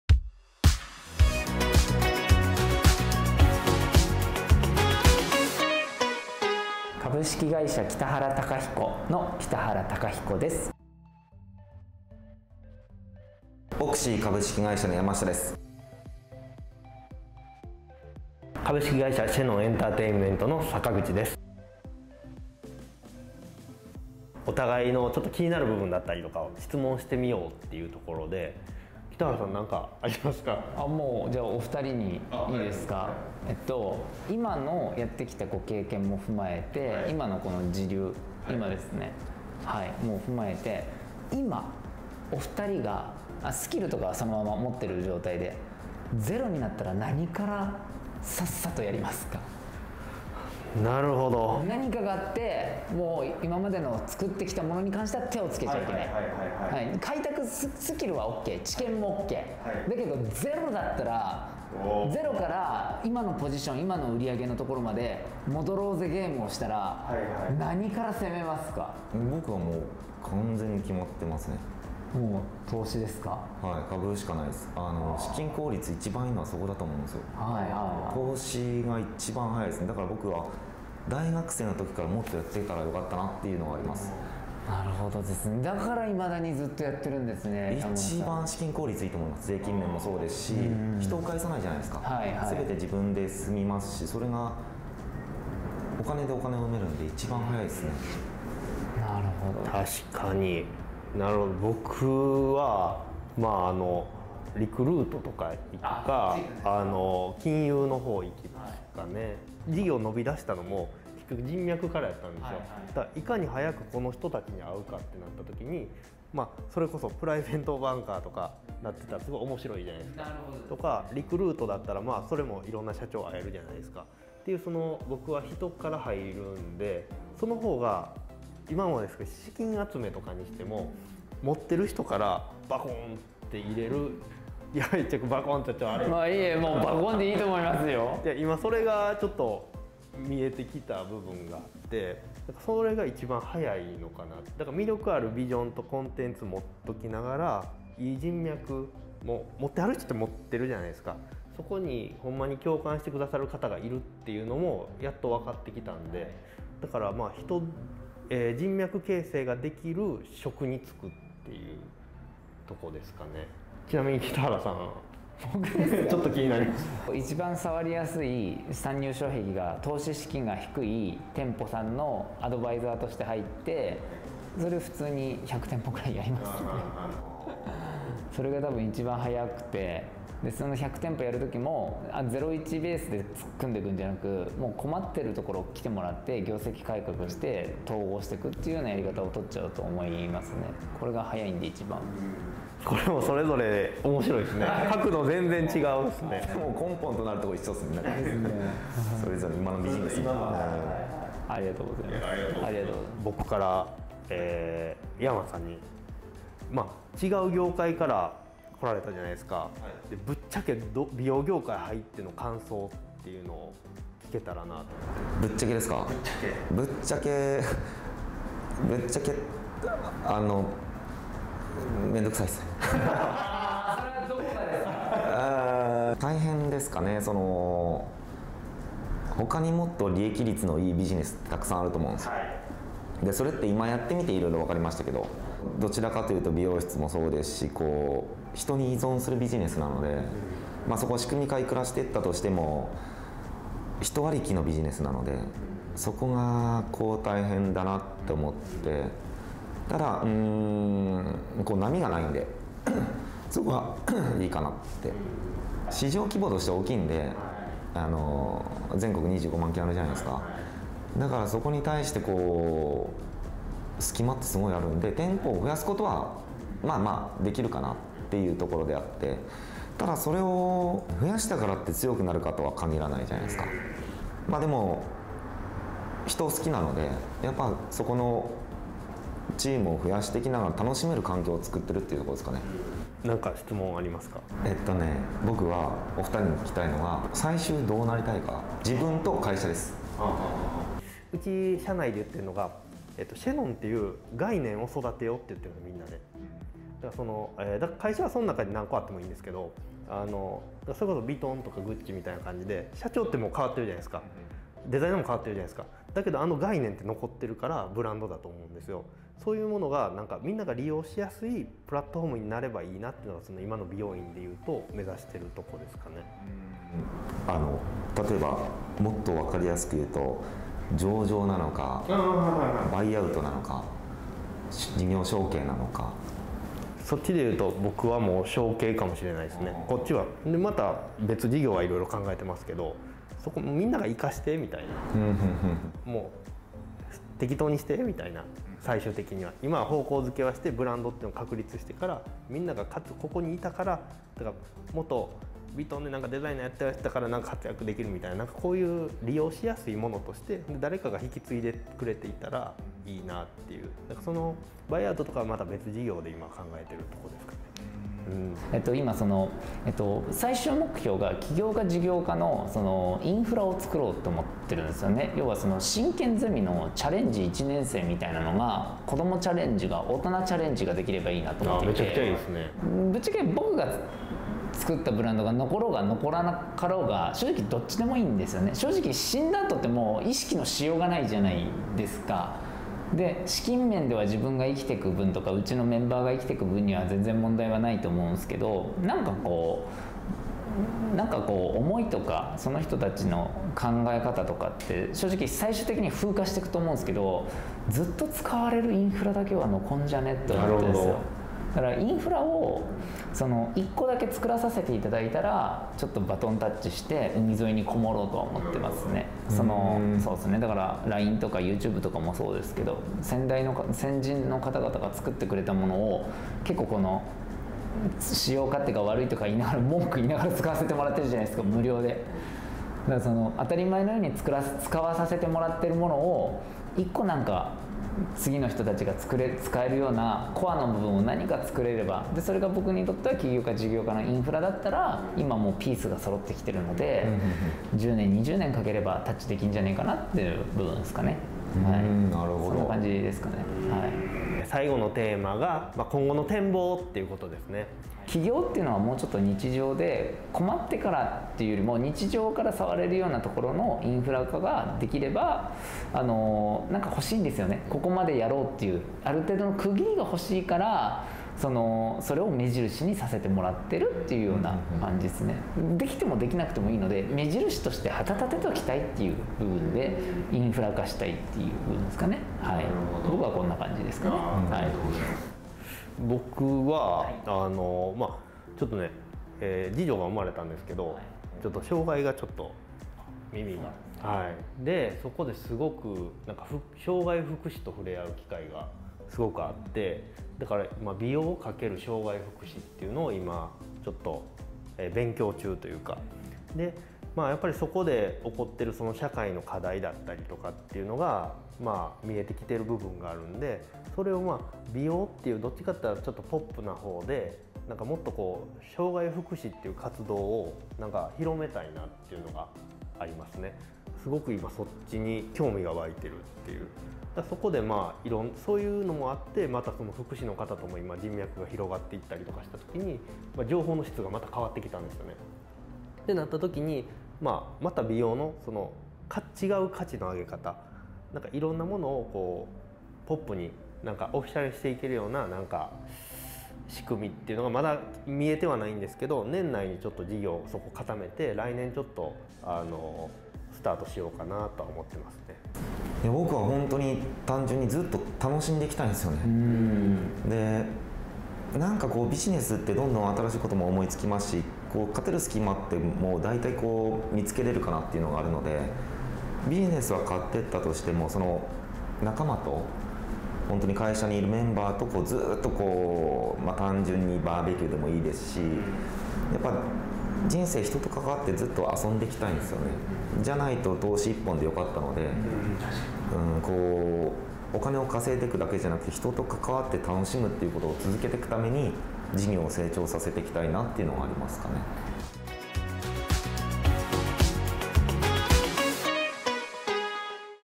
Success ─ 北原孝彦×山下拓馬×坂口貴徳 vol.4 | HAIRCAMP
経営のスペシャリストに成功の秘訣を学ぶ「Success」。
美容業界を代表する創業者・経営者同士ならではの対談を通して、美容室経営の在るべき姿を伝えます。

【 vol.4のテーマ 】
・0から今の状態に戻るなら？
・他業界から美容業界に参入してみて...
・今後の展望